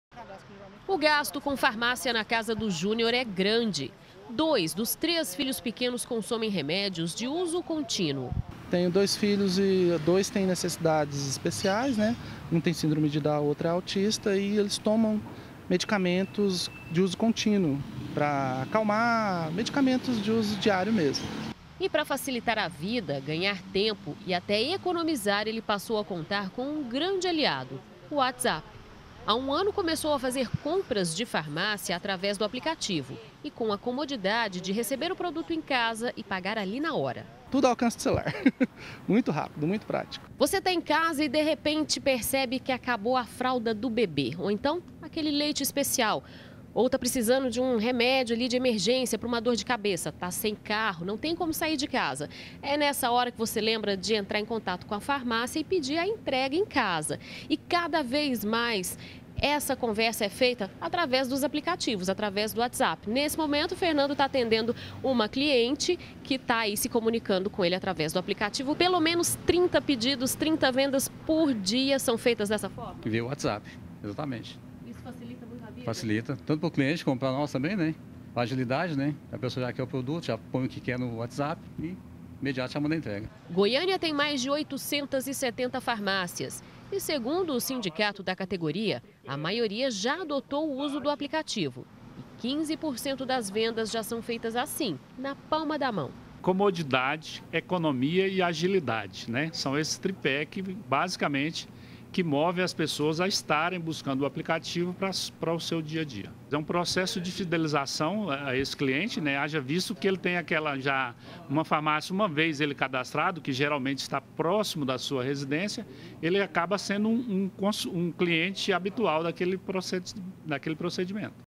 GR - Praticidade: Aplicativo para compras em farmácias - 06-02-2018
E tem crescido em 15 por cento as vendas nas farmácias de Goiânia... e isso pelo whatsapp! Um aliado na palma da mão. As pessoas estão buscando comodidade, economia e agilidade pra compras remédios ou cosméticos.